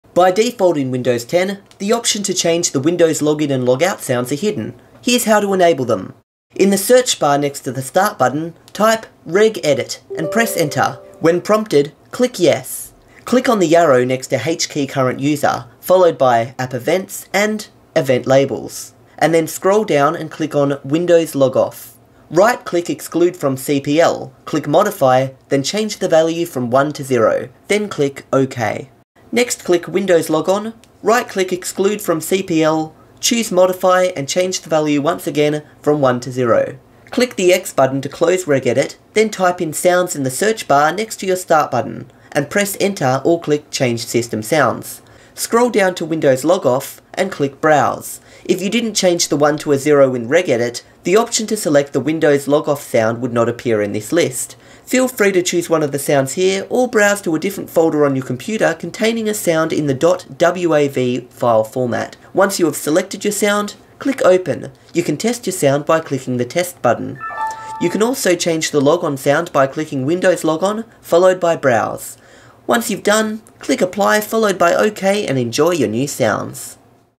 How To Quickly: Change The Windows 10 Logon/Logoff Sound
DO NOT HAVE FAST STARTUP ENABLED OR YOU WON'T HEAR THE SOUNDS YOU APPLY!

Important: Make sure you tick "Play Windows Startup Sound" otherwise this won't work! I forgot to do this in the video.

To quickly change the logon and logoff sound in Windows 10, watch this video or do the following:

1. Open Regedit
2. Go to HKEY_CURRENT_USER\AppEvents\EventLabels\WindowsLogon
3. Right click ExcludeFromCPL, click modify, then change the 1 to a 0.
4. Do the same for WindowsLogoff
5. Type "Sounds" in the windows search bar and click on "Change System Sounds".
6. Scroll down and click "Windows Log-off" or "Windows Log-on" followed by browse.
7. Choose a .wav file and click "Open".
8. Click "Play Windows Start up Sound"
9. Click "Apply" and "Ok" once you are done.